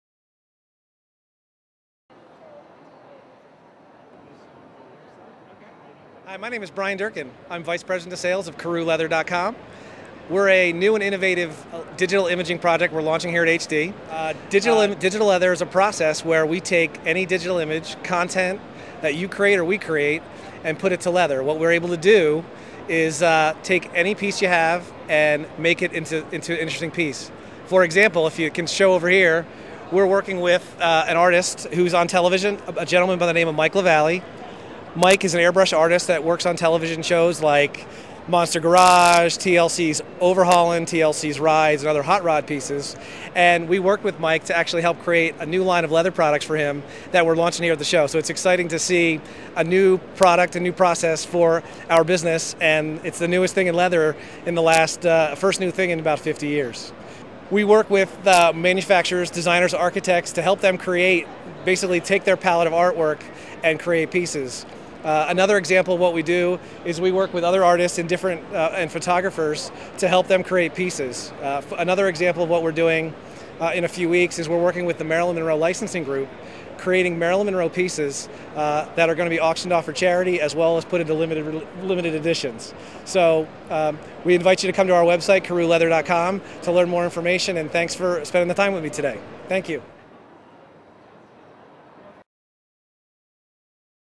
[No-Edit HD EXPO] Keru to present its leather digital imagin
Keru presented its leather digital imaging 'photography series' at the HDEXPO 2009 in Las Vegas.